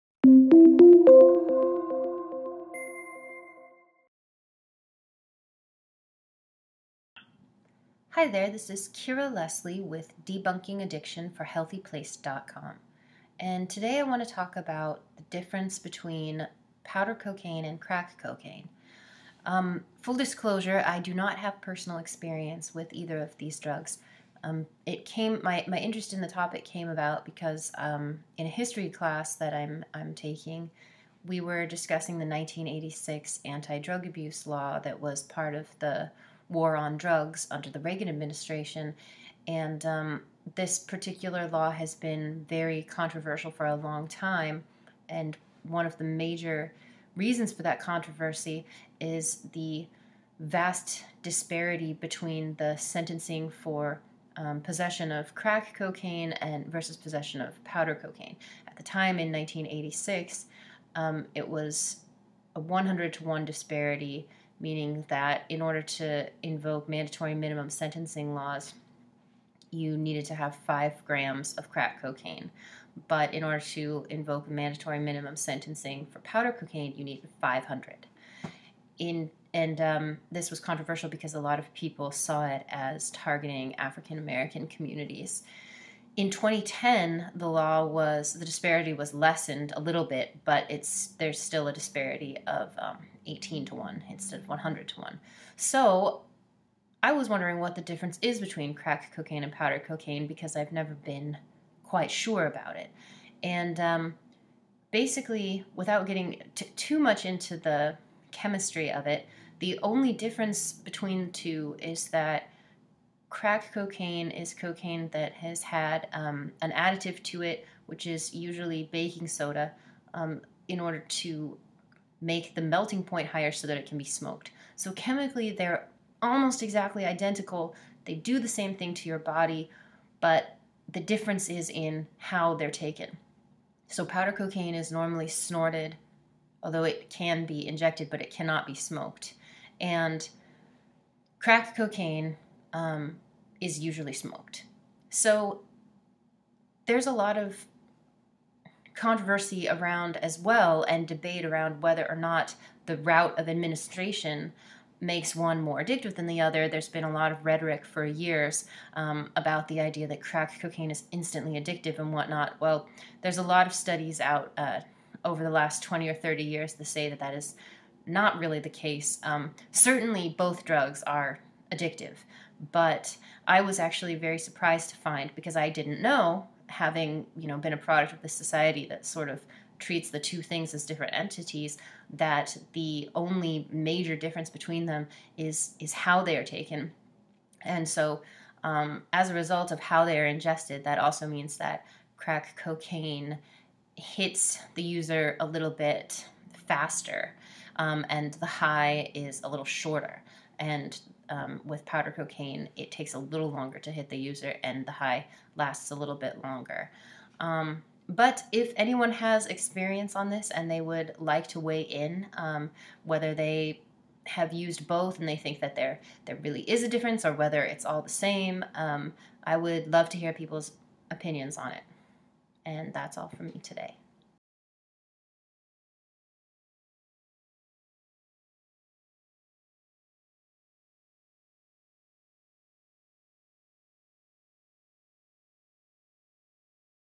Powder Cocaine vs Crack Cocaine: What's the Difference?
When you think of cocaine use, do you picture a different image than when you think of crack use? You may be surprised in the real difference between crack and cocaine. Watch.

Crack Cocaine Versus Powder Cocaine: What's the Difference?